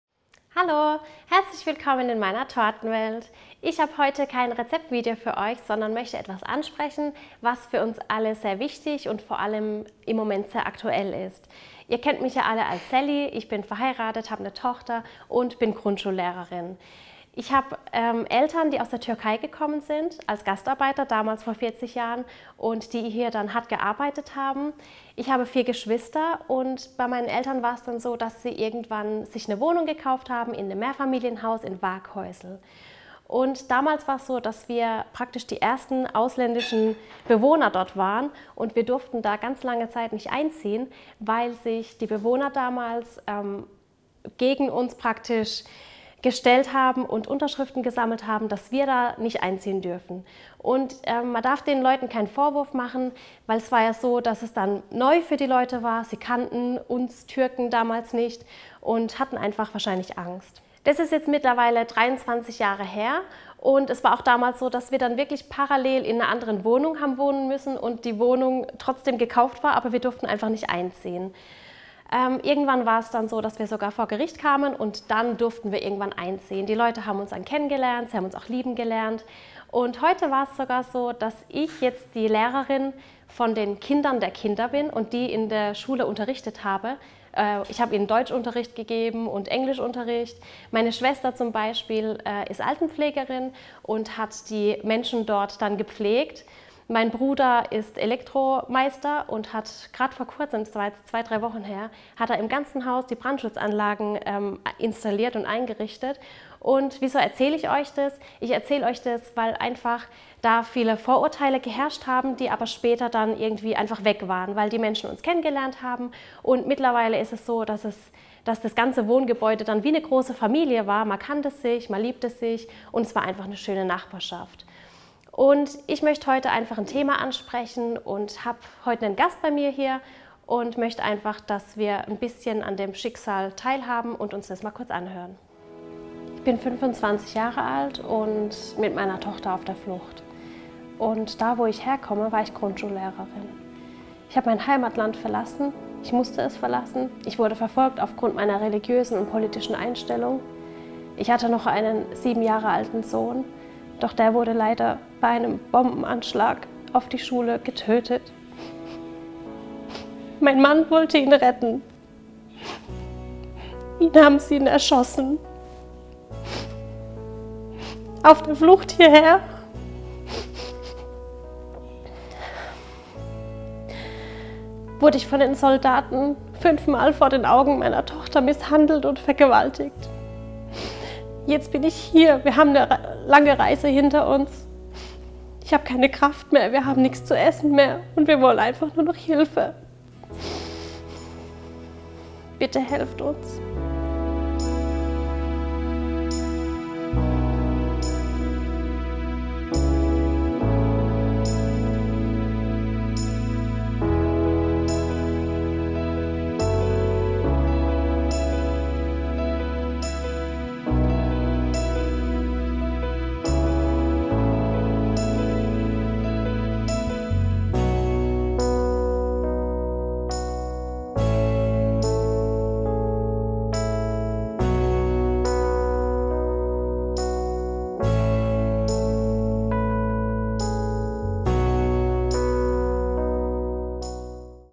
Ich bin ein Asylant ! Ich bin ein Mensch ! / Sallys Welt
Dieses Video ist der erste Teil. Im zweiten Teil werde ich zeigen, wie und wo die Spendengelder ankommen und wie dort geholfen wird.

Empfänger: Baden TV Spendenkonto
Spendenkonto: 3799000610
BLZ: 66090900

IBAN: DE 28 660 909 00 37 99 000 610
Stichwort: Aktion Sally hilft

Mit diesem Video möchte ich gerne auf die Problematik aufmerksam machen, welche uns gerade betrifft. Jeden Tag kommen neue Flüchtlinge, Asylanten in Deutschland an und suchen Unterkunft und Schutz. Wieso ist ein Land wie Deutschland nicht in der Lage diesen Menschen eine menschenwürdige Unterkunft zu bieten ?

Vor über 40 Jahren kamen meine Eltern als Gastarbeiter nach Deutschland, sie kamen freiwillig, haben hier eine Familie gegründet, gearbeitet, sich ein zu Hause aufgebaut. Meine Eltern haben sich eine Wohnung in einem großen Wohngebäude gekauft und durften über 1 Jahr nicht einziehen, weil sich die Nachbarn damals gegen uns gestellt haben. Sie haben Unterschriften gesammelt, damit die "Türken" nicht einziehen dürfen. Ein Jahr lang mussten meine Eltern in ihrer alten Wohnung Miete zahlen und parallel dazu ihre gekaufte Wohnung bezahlen. Erst nach einem Rechtsstreit durften sie einziehen. Und heute, genau 23 Jahre später, lieben die Nachbarn unsere Familie. Ich unterrichte die Enkelkinder dieser Menschen in Deutsch, meine Schwester (Altenpflegerin) pflegt die Menschen, die damals gegen unseren Einzug unterschrieben haben! Mein Bruder (Elektrikermeister) hat gerade vor kurzem die ganzen Rauchmelder im gesamten Haus installiert.

Ich mache den Menschen keinen Vorwurf, denn sie hatten damals einfach nur Angst vor dem Unbekannten, sie hatten ihre Vorurteile gegen uns "Türken".

Heute, 23 Jahre später, stehen wir hier. Nun gibt es Mitbürger, die unterschreiben, damit Asylanten nicht in unser Land, in unsere Heimat kommen. Asylanten sind Menschen, die aufgrund ihrer religiösen oder politischen Ansichten oder ihrer Rasse verfolgt werden. Viele flüchten vor dem Krieg. Diesen Menschen muss geholfen werden! Schiebt sie nicht ab in Gewerbegebiete oder Sporthallen. Das sind Menschen wie du und ich - sie haben ihre Würde. Nehmt ihnen diese Würde nicht weg. Diese Menschen sind traumarisiert, sie haben teilweise schlimmes erlebt.. Es sind gebildete Menschen, Akademiker, Lehrer, Ärzte, Männer, Frauen, Kinder. Lasst uns gemeinsam helfen.

Wir haben ein Spendenkonto eingerichtet und ich hoffe hierbei auf eure Unterstützung. Der Erlös kommt den Initiativen zugute, welche den Flüchtlingen helfen und sie unterstützen.

In der nächsten Zeit möchte ich gerne einen Tag mit den Flüchtlingskindern backen, mit ihnen einen Ausflug machen. Hierzu folgen noch Infos, ihr dürft auch hier gerne dabei sein und mit uns gemeinsam einen tollen Tag für die Kinder organisieren.

Bitte teilt mein Video, damit so viele Menschen wie nur möglich sich an der Aktion beteiligen!

Ich bedanke mich bei Baden TV für die technische Unterstützung,
bei Lisa Futterer für das Make-up,
NEW - Neues Engagement für Waghäusel

Mein großer Dank geht an alle ehrenamtliche Helfer, die sich um die Flüchtlinge kümmern.

Die Spende werde ich persönlich den Initiativen zukommen lassen, ich werde die Kamera mitnehmen und euch zeigen, wo und wie geholfen wird, hier vor Ort in Waghäusel, Bruchsal und Karlsruhe.

Weitere Infos zu Hilfsorganisationen vor Ort findet ihr bei eurer Gemeinde/Stadt.

BITTE HILF AUCH DU MIT ! NUR GEMEINSAM KÖNNEN WIR ETWAS BEWEGEN!